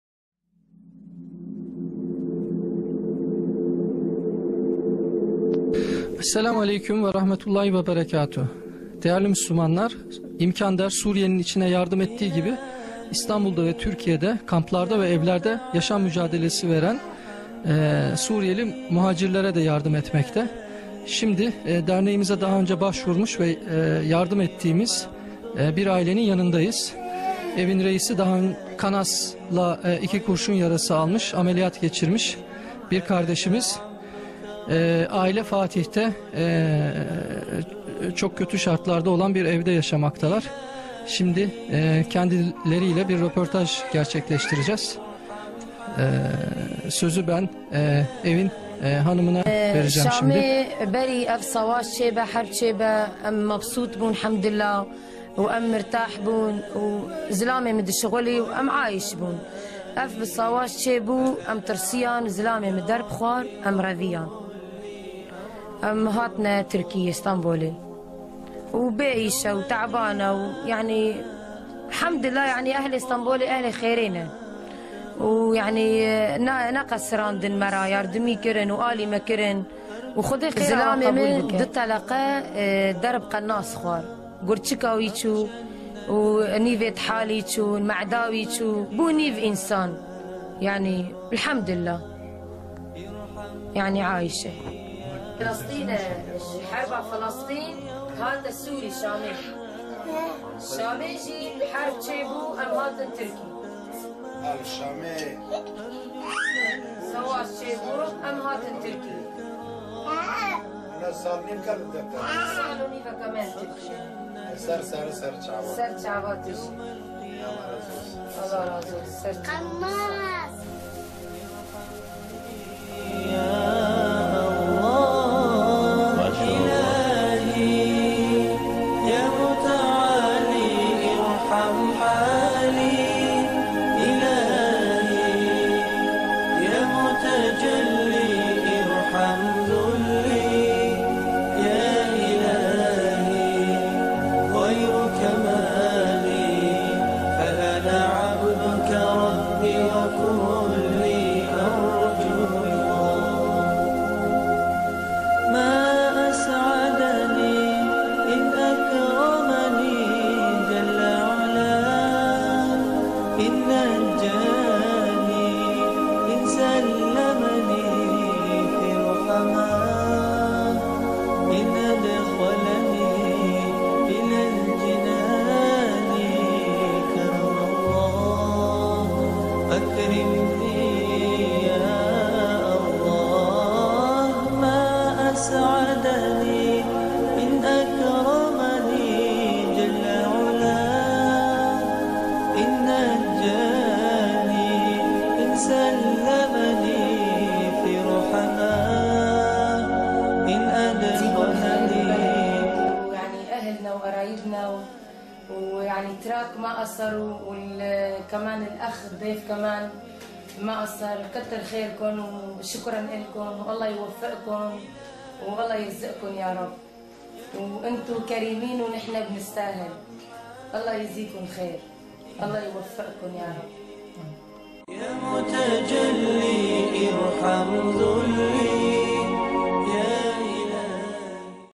Syrische Flüchtlinge in Istanbul - Reportage Teil 1 - IMKANDER
IMKANDER - Einblick in die Lebensumstände der in Not gerateten Syrischen Füchtlinge in der Türkei / Istanbul
und Notwendigkeit der finanziellen Unterstützung

"Ihr seid unsere Geschwister. Wir sind eine Familie.
Ihr seid die Großzügigen und wir sind die Bedürftigen. Möge Allah euch mit Gutem vergelten."